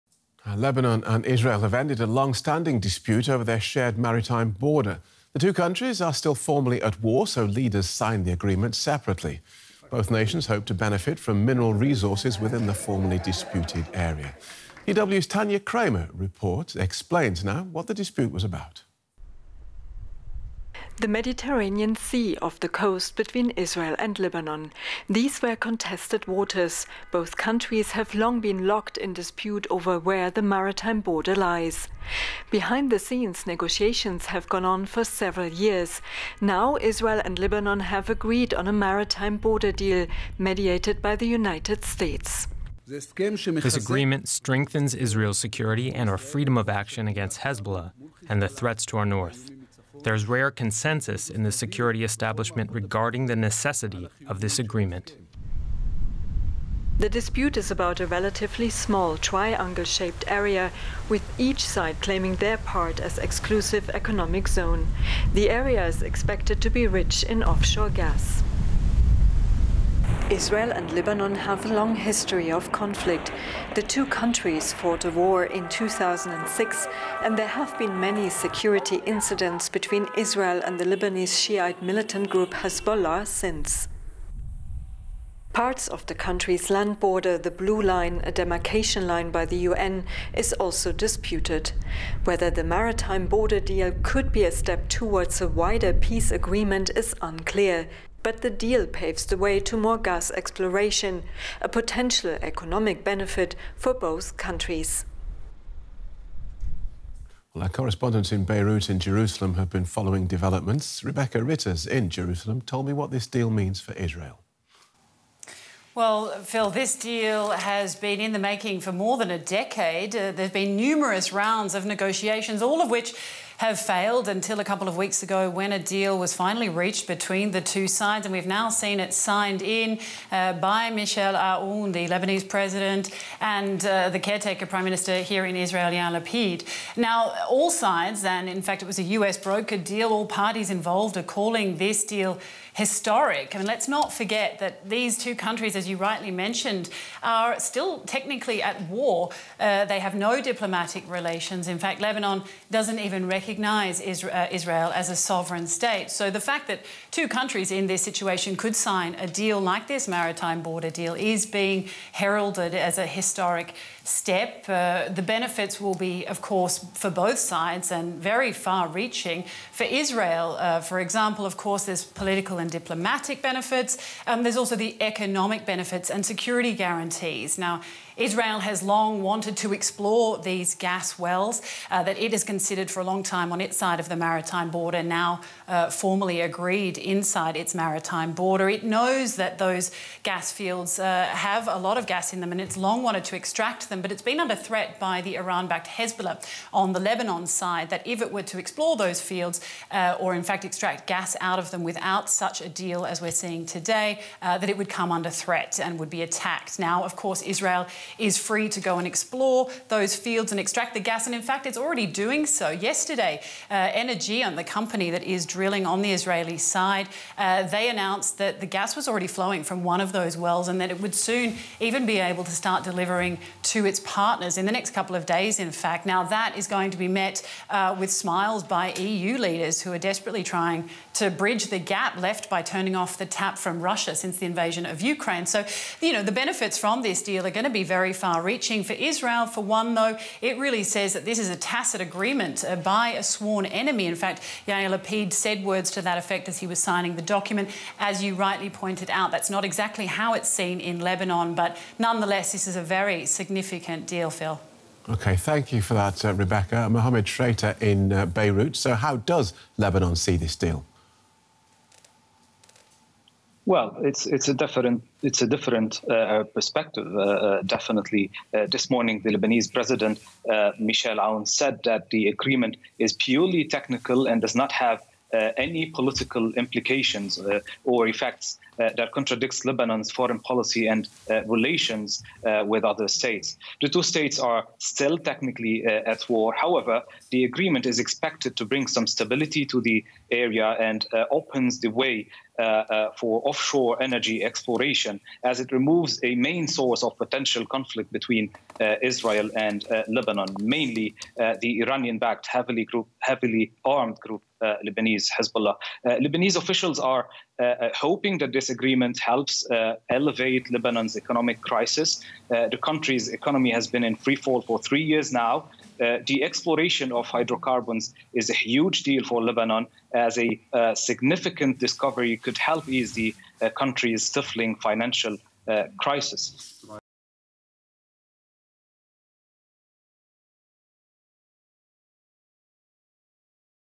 'Historic' deal brokered by the US settles maritime border between Israel and Lebanon | DW News
The deal follows months of US-brokered talks and opens up the possibility of offshore energy exploration in previously disputed waters in the eastern Mediterranean.